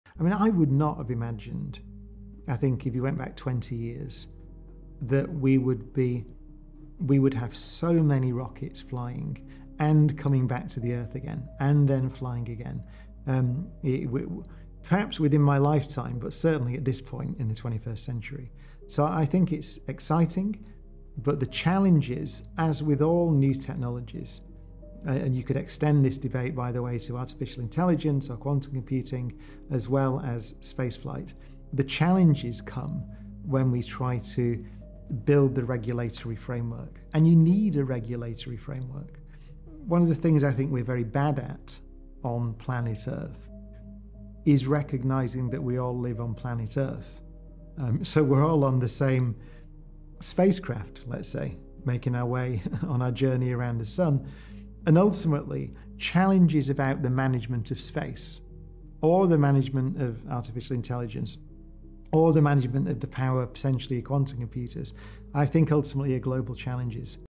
Rocket Tech Advancements & Global Challenges: The Earth Spacecraft
Reusable rockets mark exciting tech advancements, but effective global regulatory frameworks are crucial. The management of space, AI, and quantum computing power presents global challenges that require worldwide cooperation.